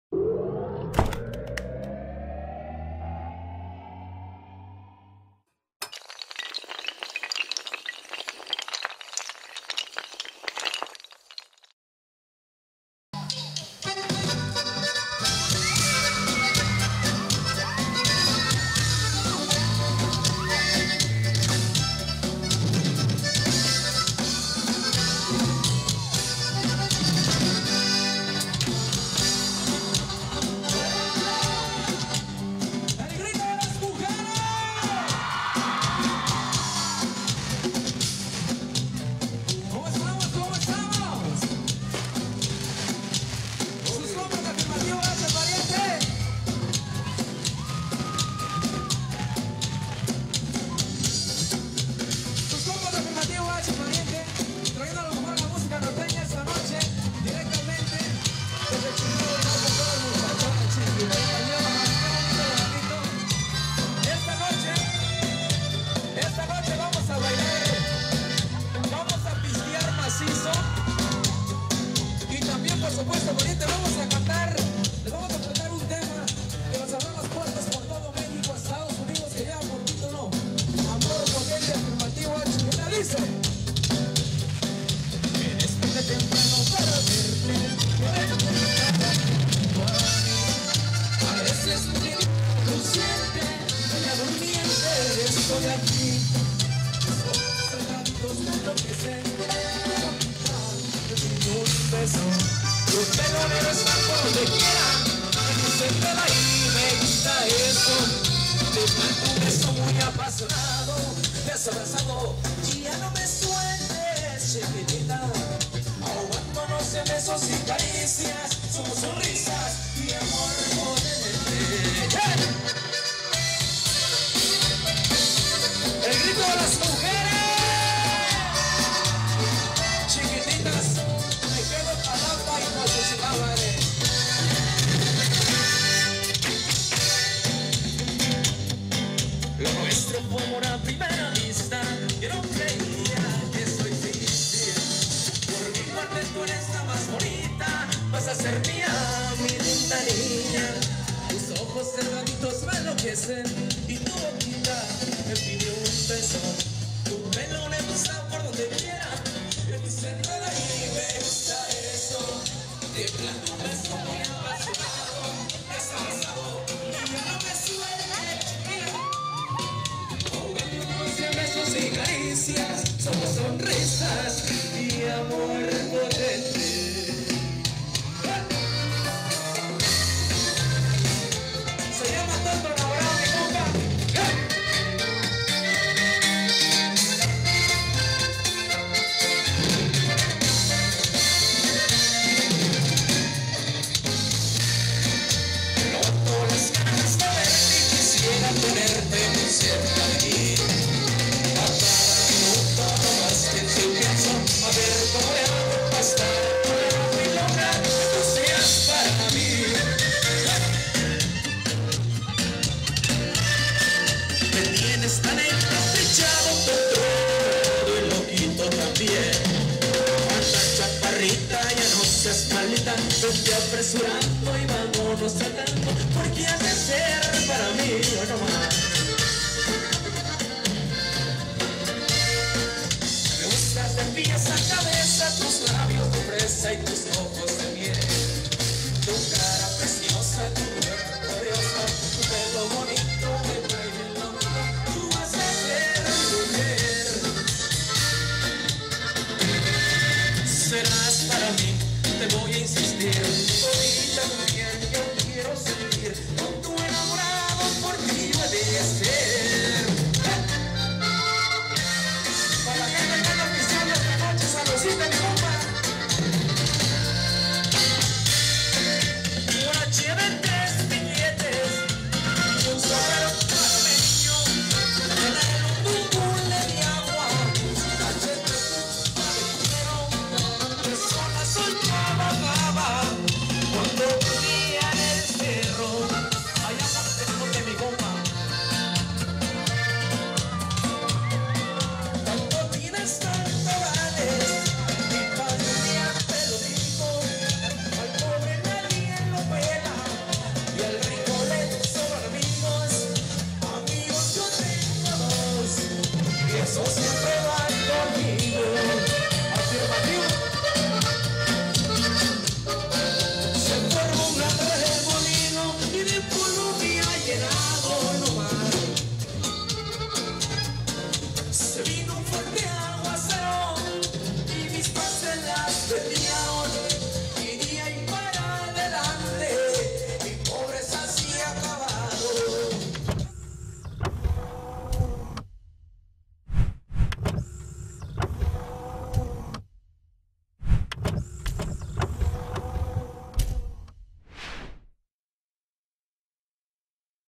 Afirmativo H, 29 aniversario de La Maquina 97.7 fm
Xalapa, Veracruz.- 06 de Octubre de 2016.-Afirmtivo H en su presentación dentro del 29 aniversario de Maquina 97.7 fm en Xalapa